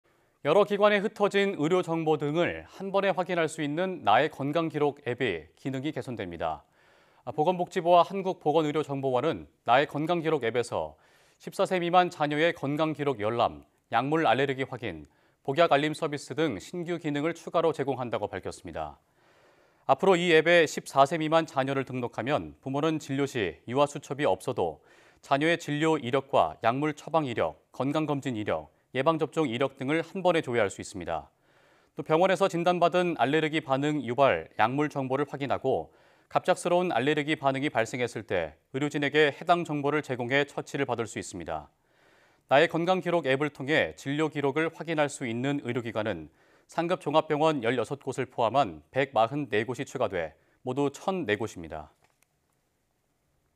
'나의건강기록' 앱으로 자녀 의료정보 한 번에 확인 / YTN 사이언스
여러 기관에 흩어진 의료 정보 등을 한 번에 확인할 수 있는 '나의건강기록' 앱의 기능이 개선됩니다.

보건복지부와 한국보건의료정보원은 나의건강기록 앱에서 14세 미만 자녀의 건강기록 열람, 약물 알레르기 확인, 복약 알림 서비스 등 신규 기능을 추가로 제공한다고 밝혔습니다.

앞으로 이 앱에 14세 미만 자녀를 등록하면 부모는 진료 시 유아 수첩이 없어도 자녀의 진료 이력과 약물 처방 이력, 건강검진 이력, 예방접종 이력 등을 한 번에 조회할 수 있습니다.

또 병원에서 진단받은 알레르기 반응 유발 약물 정보를 확인하고 갑작스러운 알레르기 반응이 발생했을 때 의료진에게 해당 정보를 제공해 처치를 받을 수 있습니다.

나의건강기록 앱을 통해 진료 기록을 확인할 수 있는 의료기관은 상급종합병원 16곳을 포함한 144곳이 추가돼 모두 1,004곳입니다.